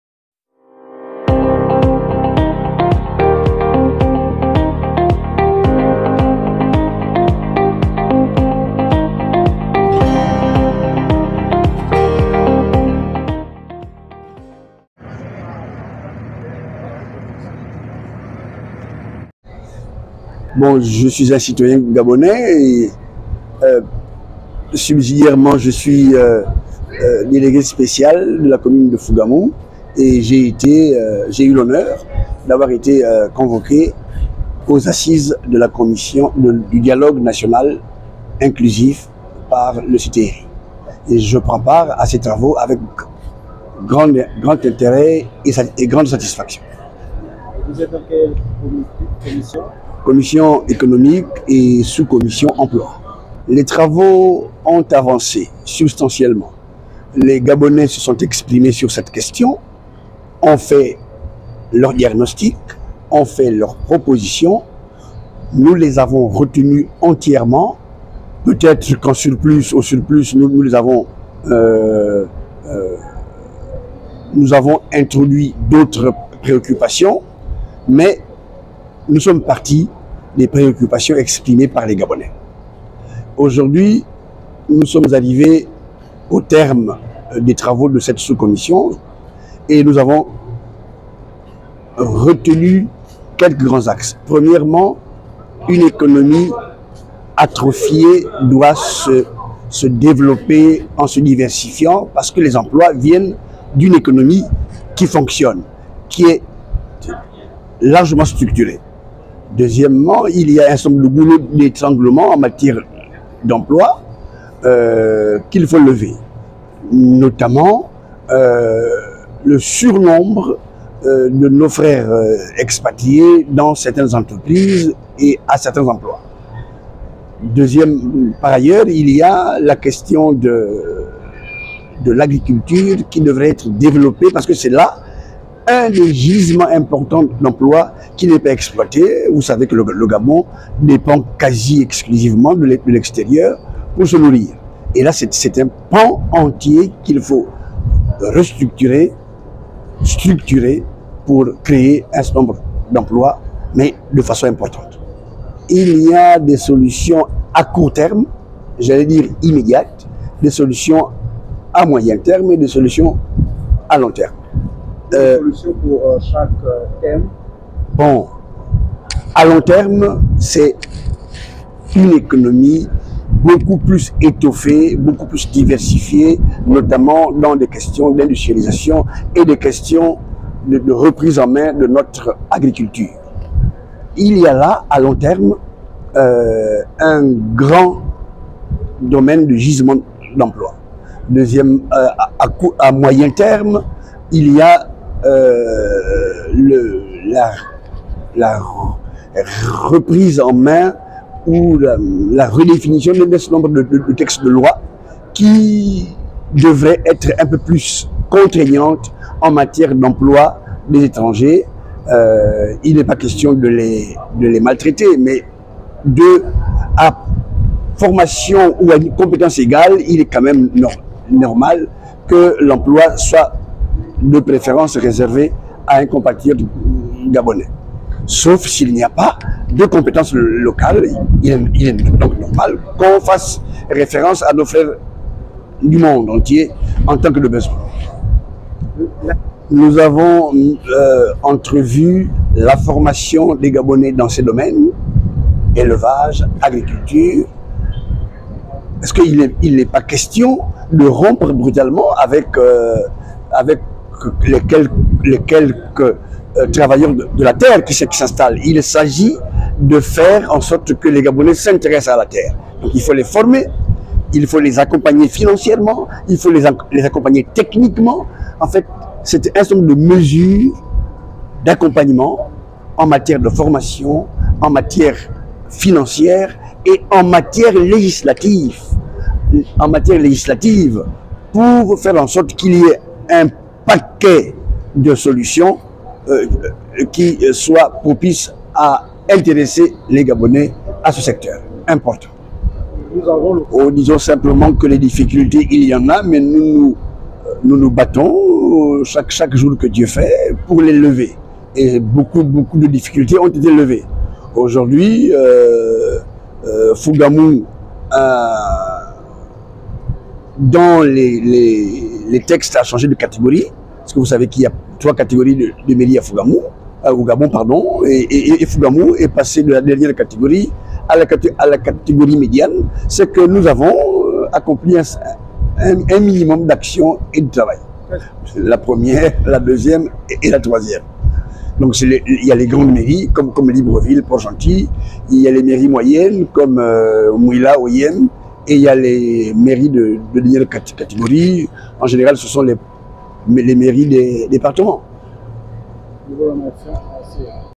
DNI: Le surnombre des expatriés aux postes des Gabonais doit être résolu rappelle Moutandou-Mboumba
Lors d'un entretien exclusif du Délégué spécial de la commune de Fougamou, Joseph Moutandou-Mboumba, il a rappelé le fait que la sous Commission à laquelle il appartient a  exprimé le fait que le problème du surnombre de nos frères expatriés qui occupent les postes des Gabonais doit être résolu rapidement comme commencement de la résolution du chômage.